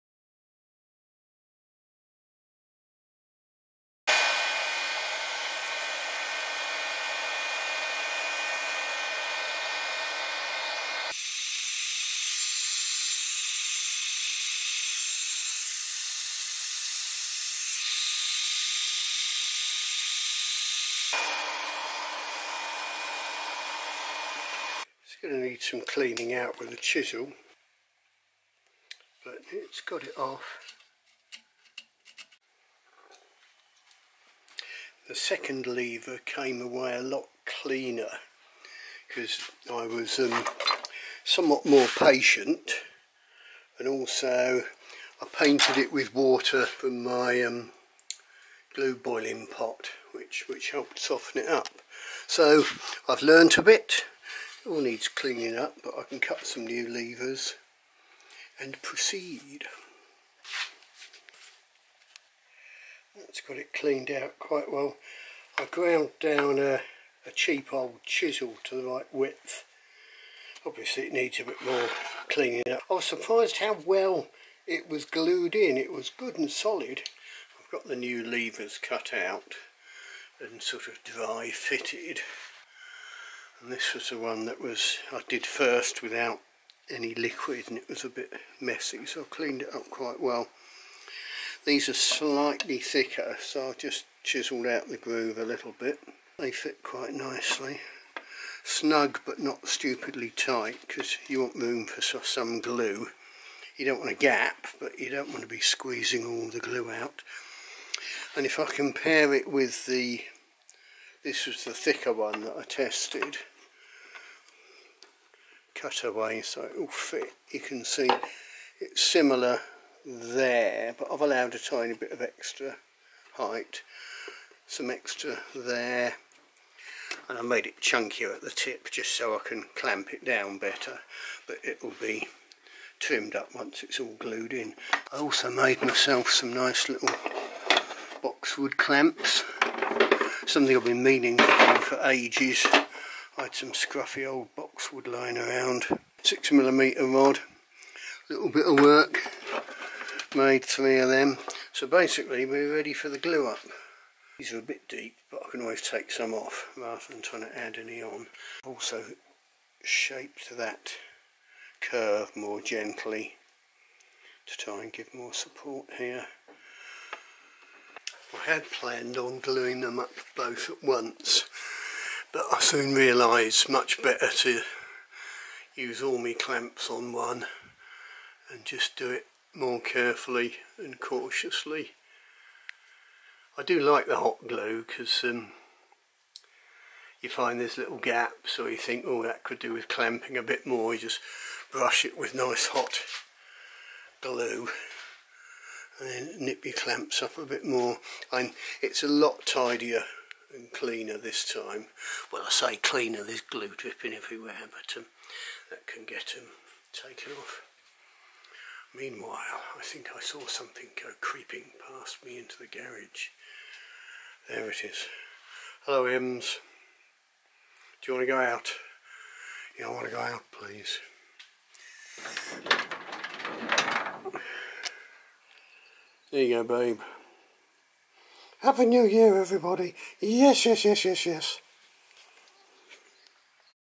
Hickory Flight Bow rework pt6
Gluing in the new levers. I'm a bit anxious that I've made the glue too thin (the 'pearls' are covered with water, left over night to absorb it, then warmed up). So I glued up a couple of small scraps as test pieces.